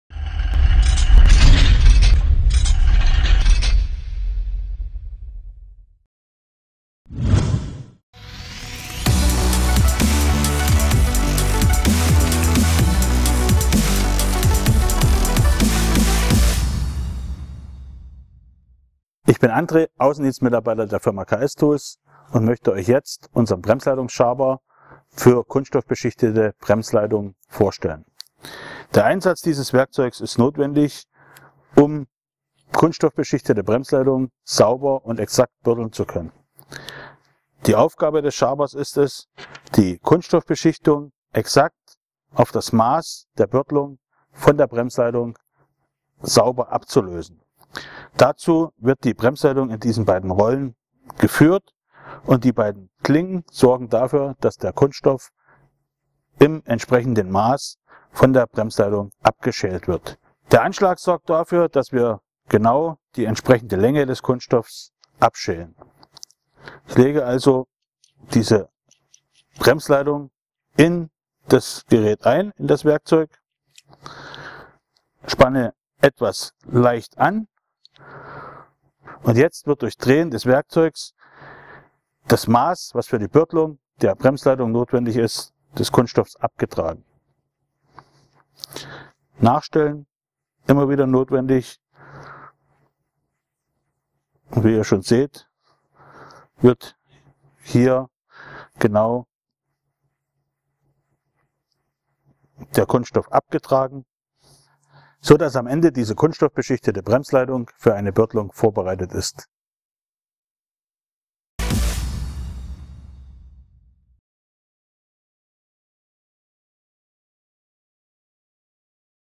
KS Tools | 150.2226 - Bremsleitungsschaber 3-16 mm by GÖHRUM Fahrzeugteile GmbH
• ideal geeignet zum Entfernen der zusätzlichen Ummantelung bei neuartigen Bremsleitungen
• ermöglicht eine bessere Bördelqualität
• 2 Schabkanten
• mit Anschlag für die genaue Positionierung der Bremsleitung
• ergonomisch geformt
• für Kupfer-, Aluminium- und Stahlleitungen
• mit gerändeltem Drehknopf
• Metallgehäuse

Anwendungsgebiete:
Audi, BMW, Opel, Seat, Škoda und Volkswagen mit ummantelten Bremsleitungen

▬ GÖHRUM ▬▬▬▬▬▬▬▬▬▬▬▬▬▬▬

▬ Die Fakten ▬▬▬▬▬▬▬▬▬▬▬▬▬▬

EIN TEIL DES ERFOLGS:
- Handelstreue
- klare Sortimentspolitik

GÖHRUM Fahrzeugteile GmbH
Ulmenstr. 19
71069 Sindelfingen

▬ Ende ;-) ▬▬▬▬▬▬▬▬▬▬▬▬▬▬▬